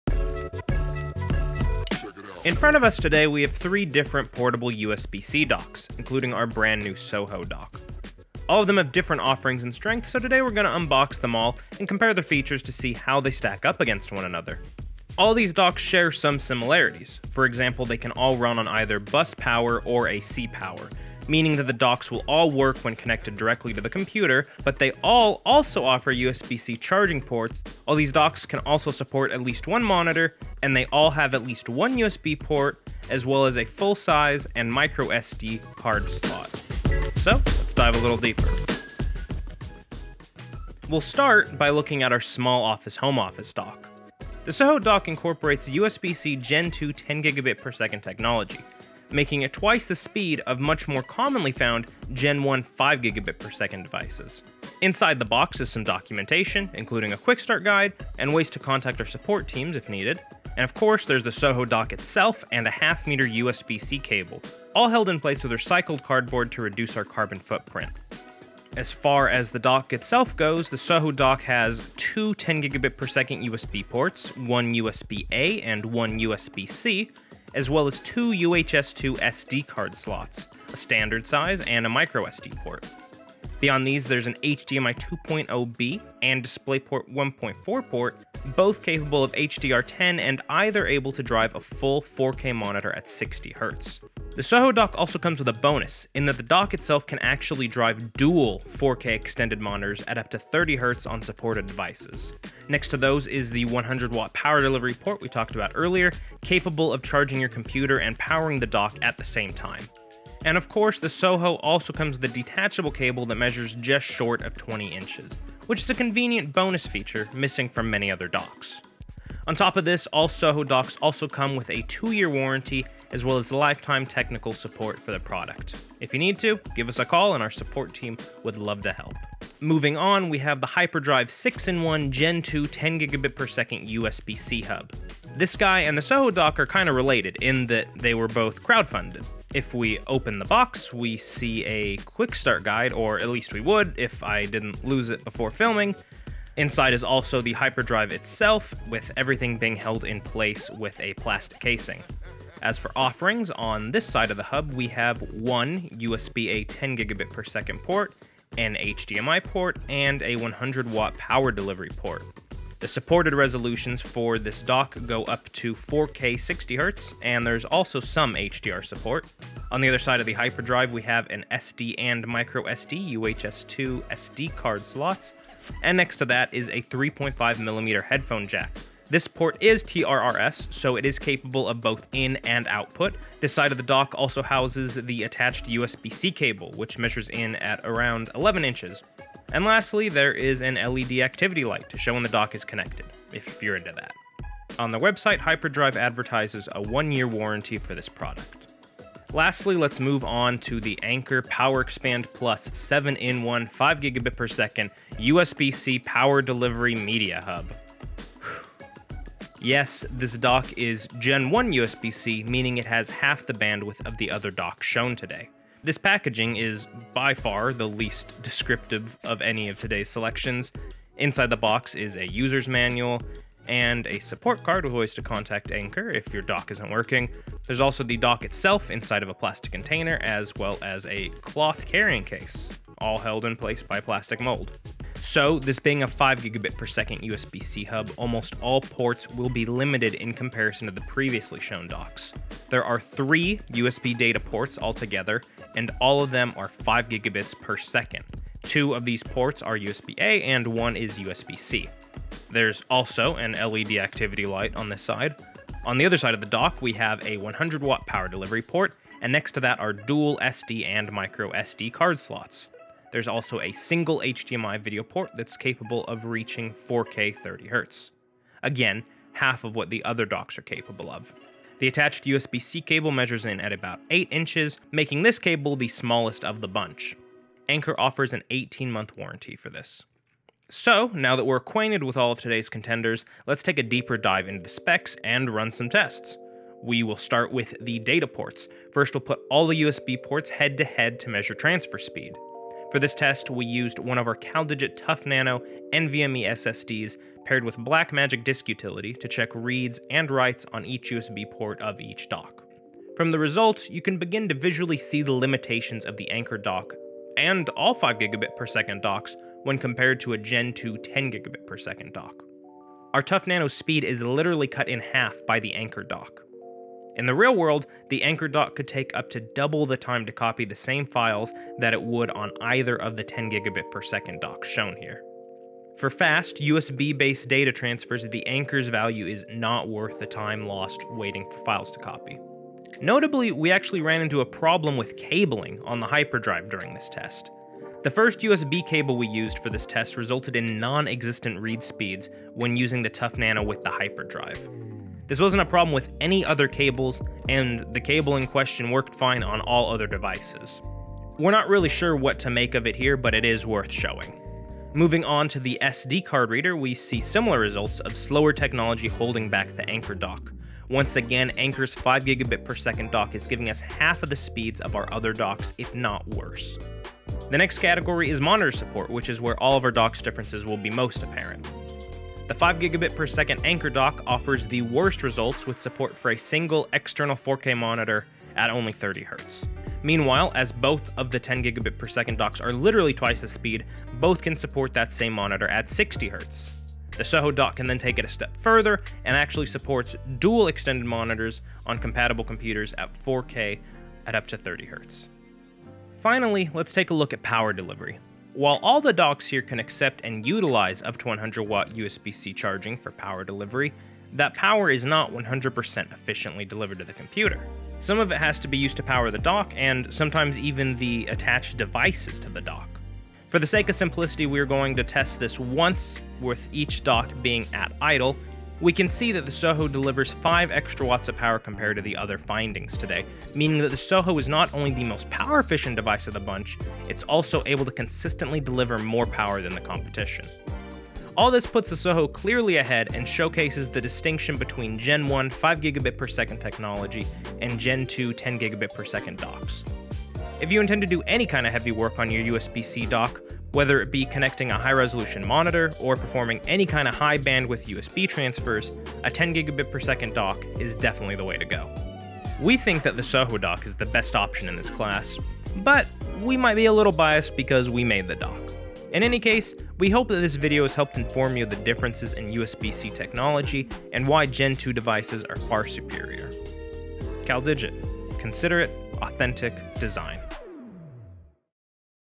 SOHO Dock - USB-C Dock Showdown
How does the SOHO Dock stack up against the competition?  We decided to put our dock to the test.  Here's our results.

See the SOHO Dock's maximum supported resolution based on your macOS computer

0:00 - Intro
0:40 - SOHO Dock Unboxing
2:09 - HyperDrive GEN2 6-Port USB-C Hub Unboxing
3:19 - Anker PowerExpand+ 7-in-1 USB-C Hub Unboxing
4:40 - Benchmarks
4:48 - USB Benchmarks
5:55 - SD Benchmarks
6:07 - Monitor Support
6:35 - Power Delivery Benchmarks
7:11 - Summary